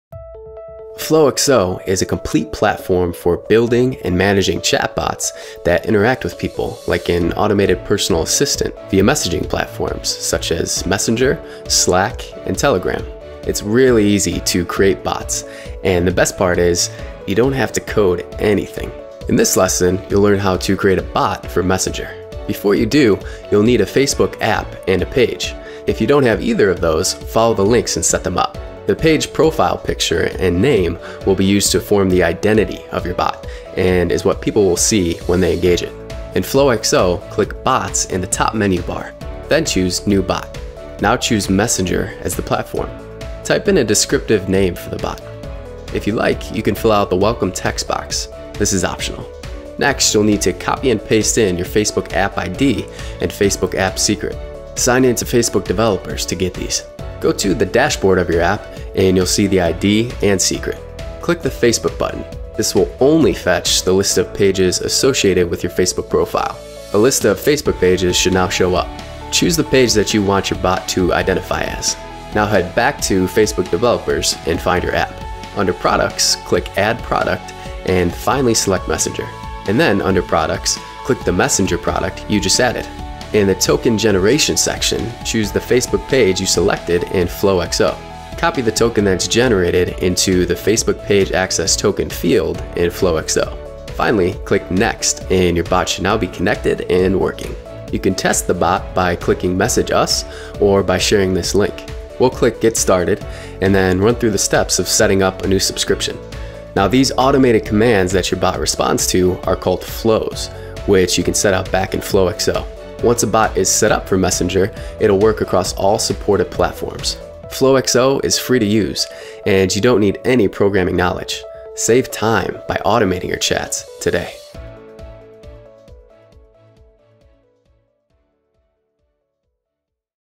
Setting up a Messenger Bot with Flow XO
See how easy it is to set up a Facebook Messenger bot with Flow XO, the powerful visual bot builder.